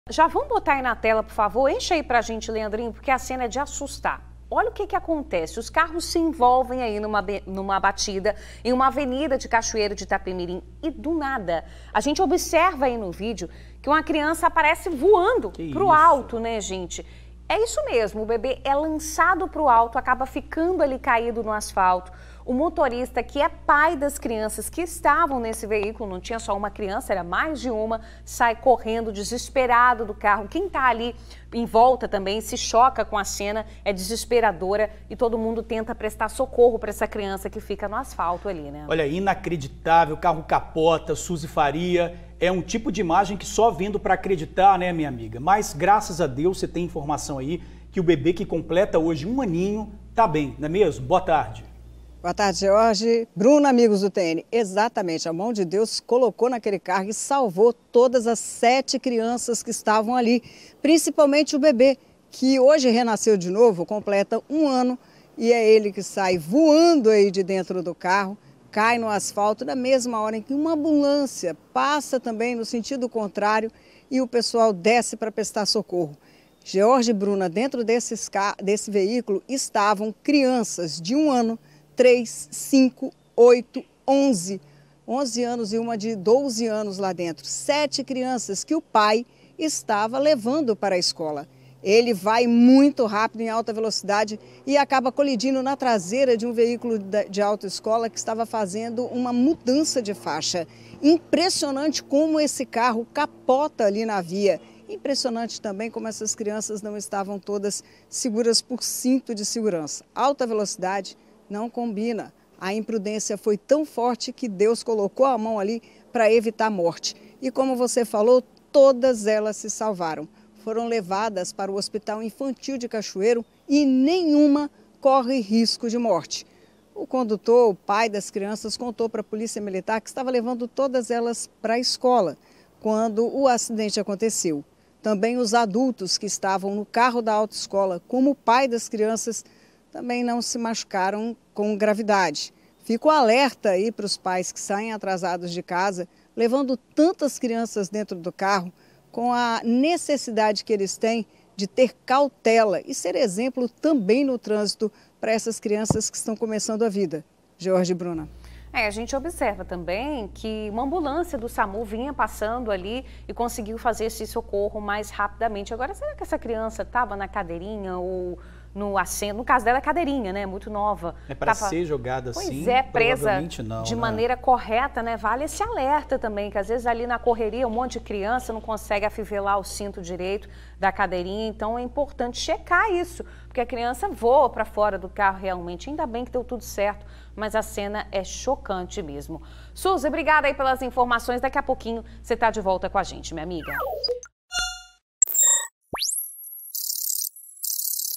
Criança é arremessada de carro em acidente no ES
Ela foi socorrida e levada ao hospital após o acidente em Cachoeiro de Itapemirim, no Sul do Espírito Santo.